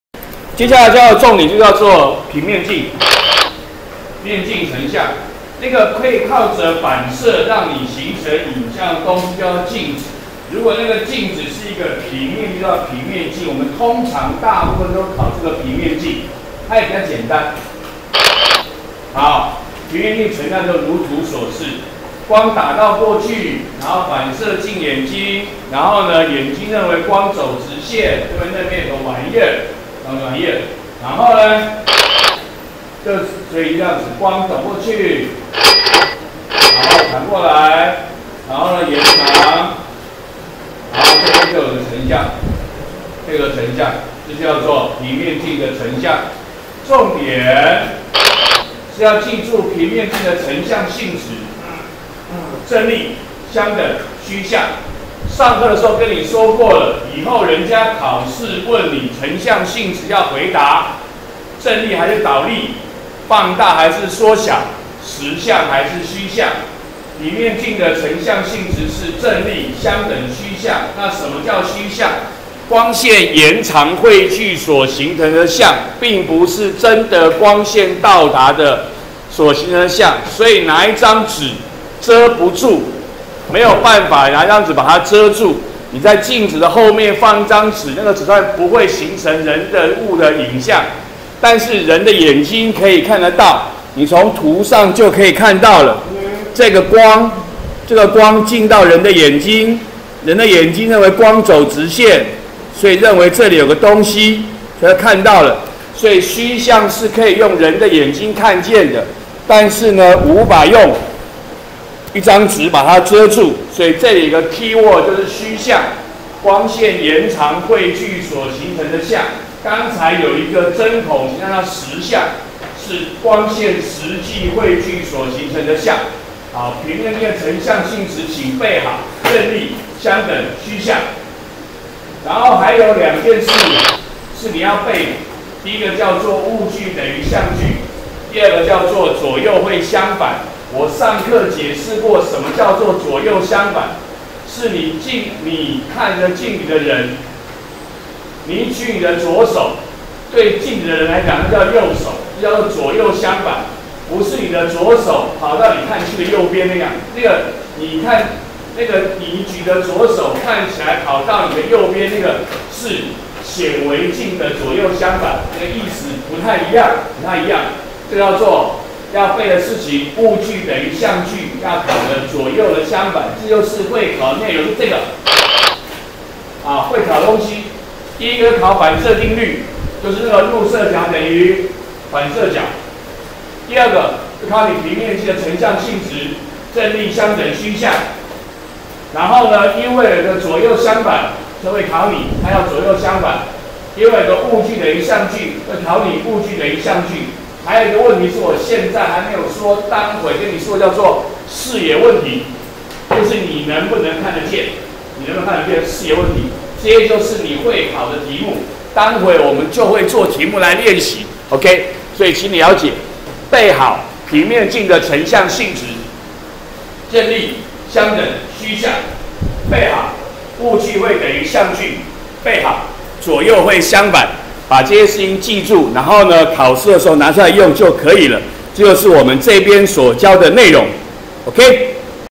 103學年複習光05：平面鏡成像與成像性質
介紹鏡子及平面鏡。說明平面鏡的成像及成像性質，以及另外兩個需要背的重點。最後提示會考的題型。